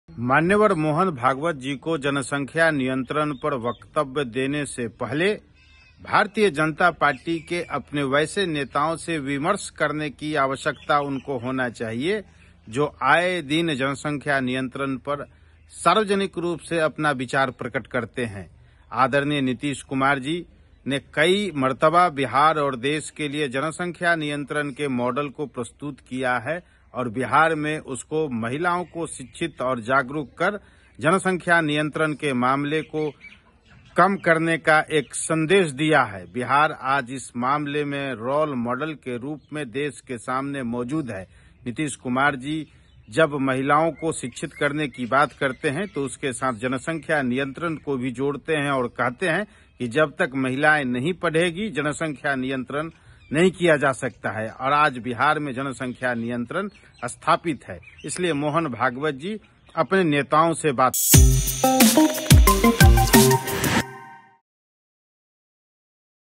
Mohan Bhagwat के बयान पर JDU नेता Arvind Nishad ने दे दी नसीहत, कहा- पहले BJP से विमर्श करने...'
मोहन भागवत को जनसंख्या नियंत्रण पर वक्तव्य देने से पहले भारतीय जनता पार्टी के नेताओं से विमर्श करने की आवश्यकता होना चाहिए. बीजेपी के नेता सार्वजनिक रूप से आय दिन जनसंख्या नियंत्रण पर अपना राय प्रकट करते हैं. मुख्यमंत्री नीतीश कुमार बिहार और देश के लिए जनसंख्या नियंत्रण पर मॉडल पेश किया है. जनसंख्या नियंत्रण के मामले में बिहार ने एक संदेश दिया है जो रोल मॉडल के रूप में देश के सामने मौजूद है. नीतीश कुमार ने महिला को शिक्षित करने की बात कह कर जनसंख्या को नियंत्रित किया है